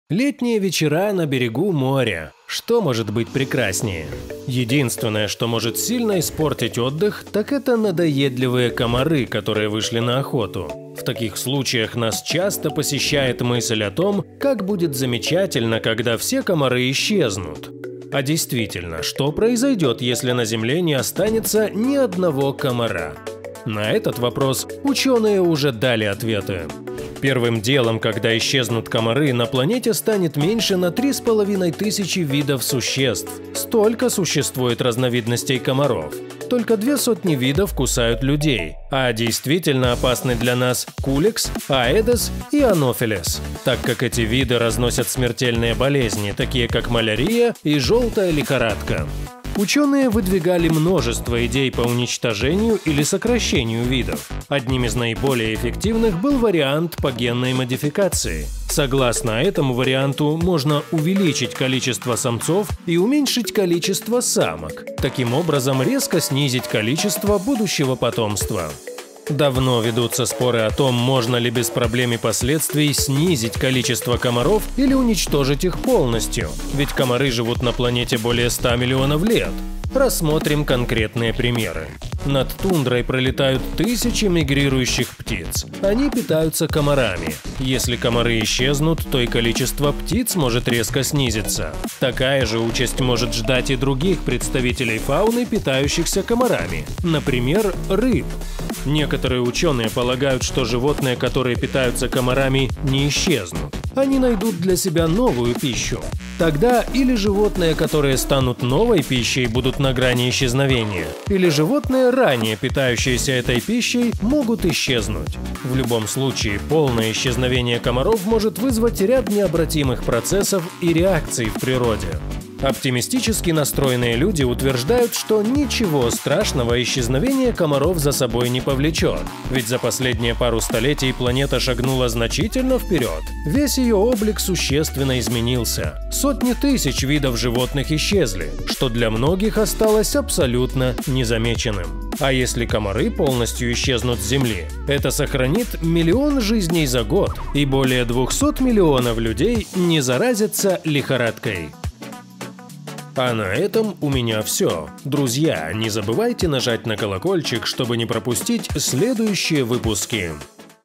▽ Что было, если бы исчезли все комары на планете
Летние вечера на берегу моря, что может быть прекраснее? Единственное, что может сильно испортить отдых, так это надоедливые комары, которые вышли на охоту. В таких случаях нас часто посещает мысль о том, как будет замечательно, когда все комары исчезнут. А действительно, что произойдет, если на Земле не останется ни одного комара? На этот вопрос ученые уже дали ответы.

Создано живыми людьми! Мы не используем искусственный интеллект (ИИ)